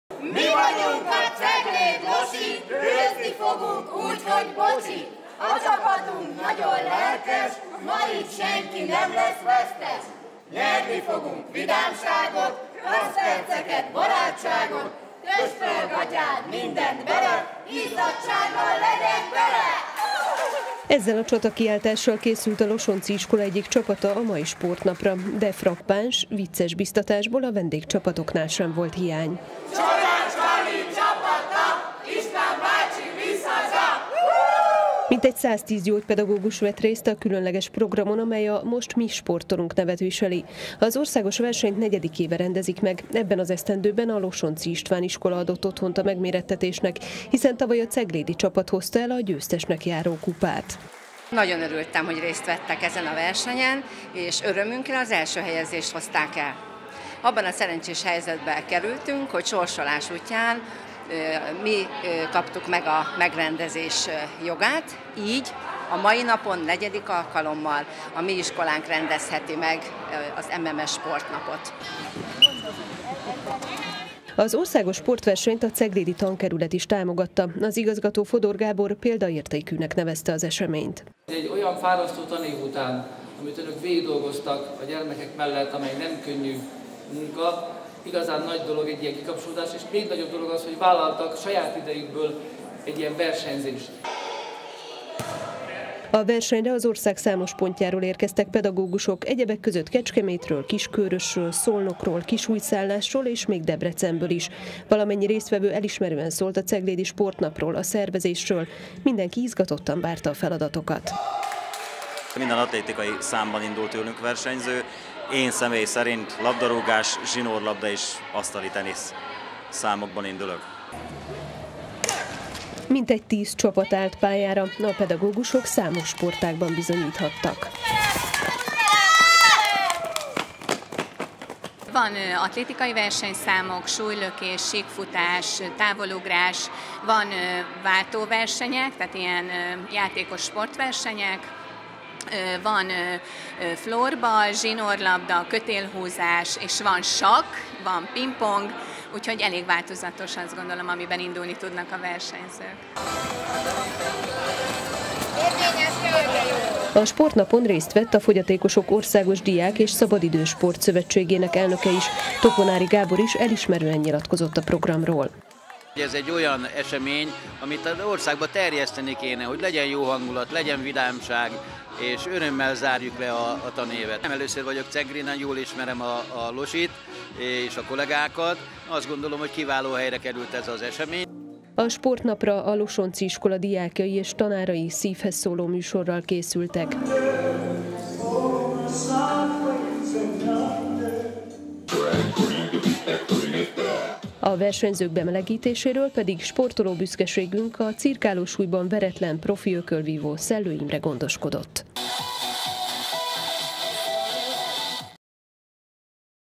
Gyógypedagógusok versenyeztek ma a Losontzi István Iskolában
Gyógypedagógusok versenyeztek ma a Losontzi István Iskolában. Az intézmény adott otthont annak a megmérettetésnek, amelyben a hazai gyógypedagógia iskolák tanárait várták jó hangulatú sportnapra. Az eseményen az ország számos pontjáról több mint tíz csapat vett részt. 110 pedagógus mérte össze tudását fociban, súlylökésben, távolugrásban, de a versenyszámok között volt sakk, pingpong és kötélhúzás is.